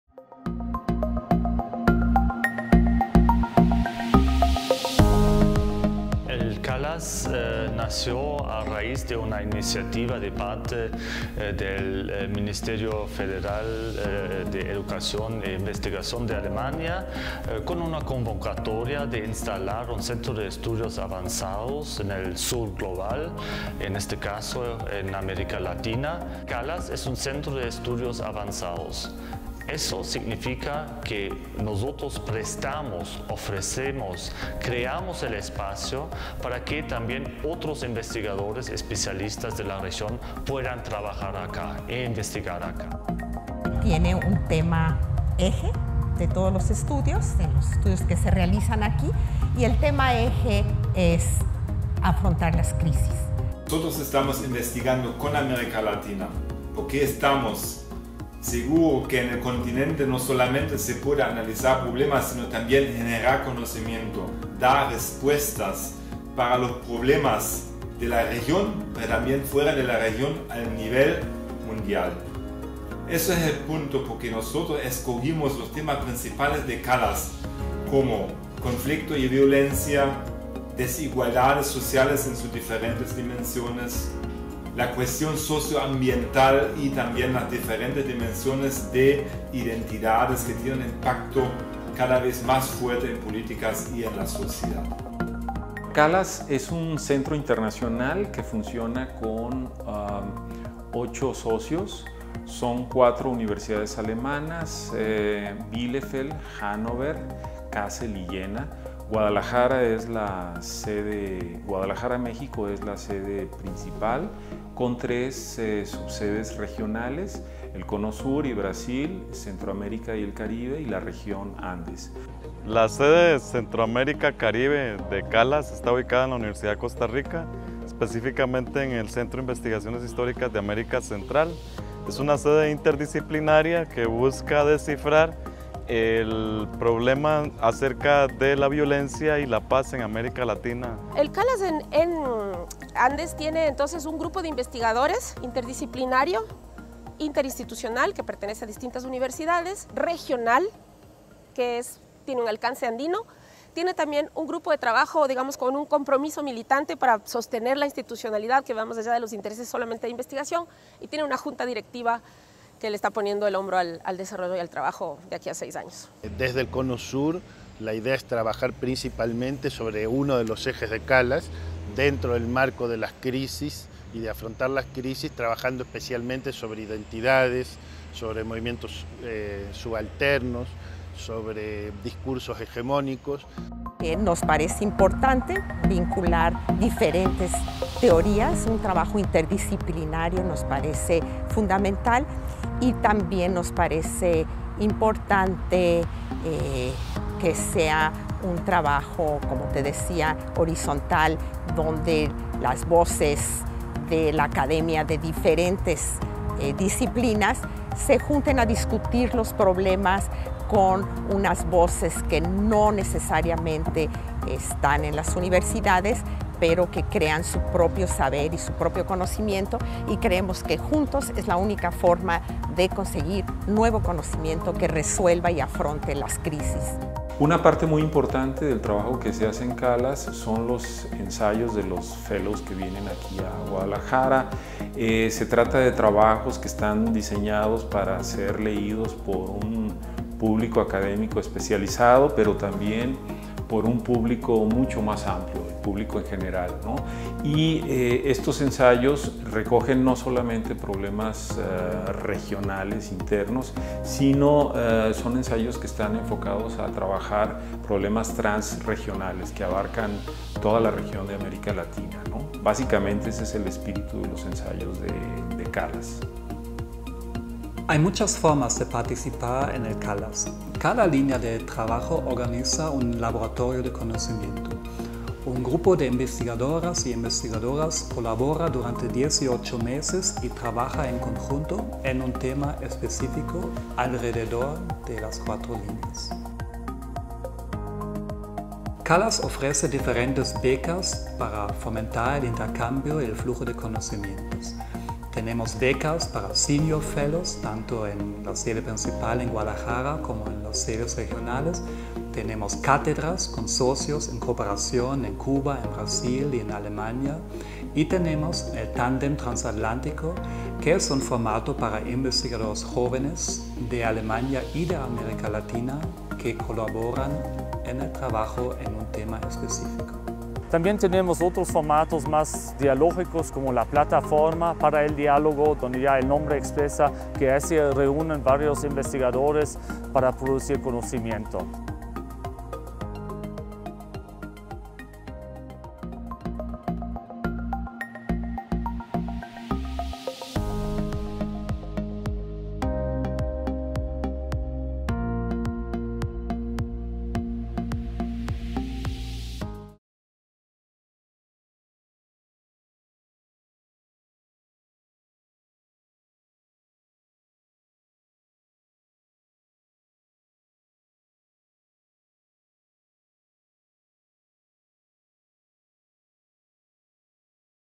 Introducing CALAS
El Centro Maria Sibylla Merian de Estudios Latinoamericanos Avanzados (CALAS) es un espacio académico que se dedica a fortalecer la cooperación académica transdisciplinaria y transregional en las Ciencias Sociales y Humanidades entre América Latina y Alemania.  El CALAS invita a investigadoras e investigadores en generar nuevo conocimiento en torno al tema «Afrontar las crisis: perspectivas transdisciplinarias desde América Latina». Reúne a diferentes disciplinas, nacionalidades, generaciones y campos sociales, creando un entorno productivo para la investigación innovadora y comprometida fundamentada en el diálogo, el respeto mutuo y el intercambio libre de ideas.